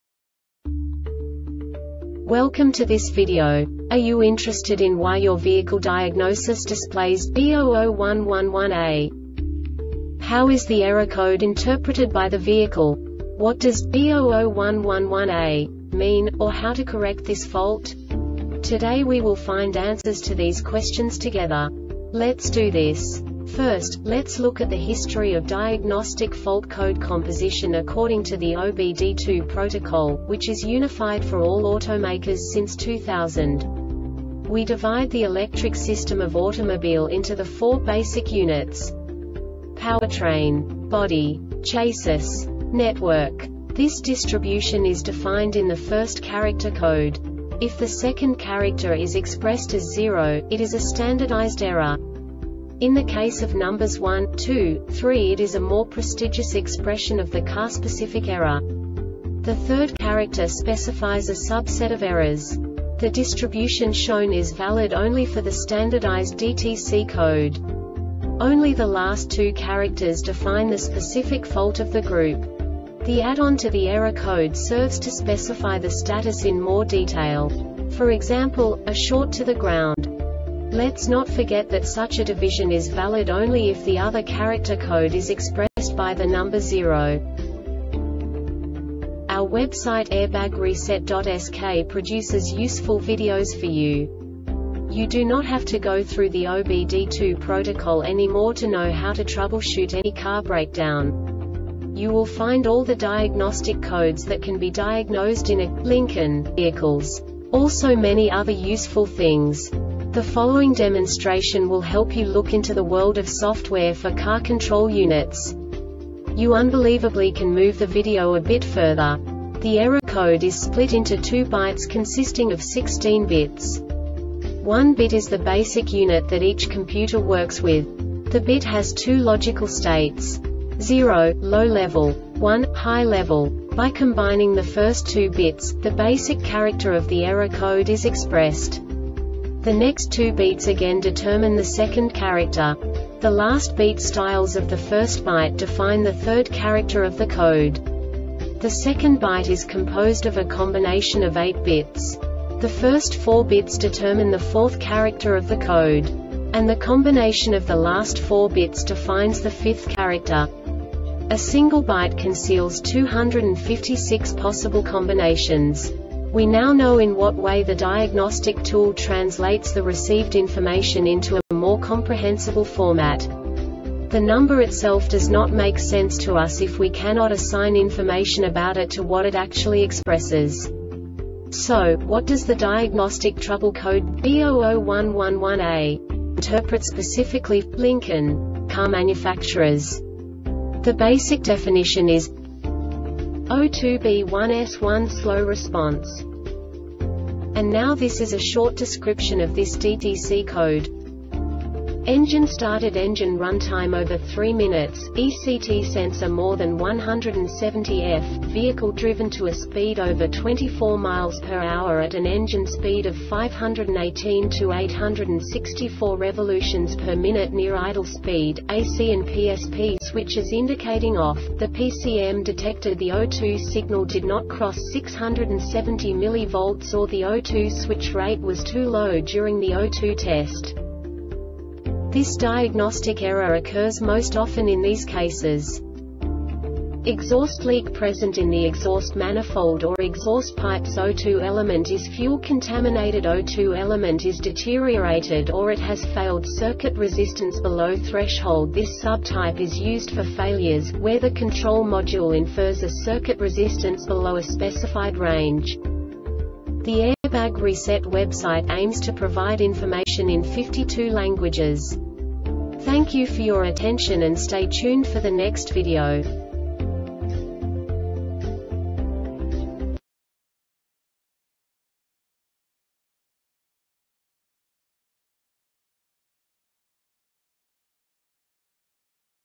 DTC Lincoln B0011-1A Short Explanation
Contents:
0:21 Basic DTC analysis according to OBD2 protocol standard.
1:48 Insight into programming
2:58 A diagnostically understandable description of the Diagnostic Trouble Code
3:05 Basic error definition

Make:
Lincoln
Code:
B0011 1A
Definition:
O2 (B1 S1) Slow Response
Description:
Engine started;engine runtime over 3 minutes,ECT Sensor more than 170ºF,vehicle driven to a speed over 24 mph at an engine speed of 518-864 rpm (near idle speed),A or C and PSP switches indicating off,the PCM detected the O2 signal did not cross 670 mV or the O2 switch rate was too low during the O2 Test.
Cause:
Exhaust leak present in the exhaust manifold or exhaust pipesO2 element is fuel contaminatedO2 element is deteriorated or it has failed
Failure Type: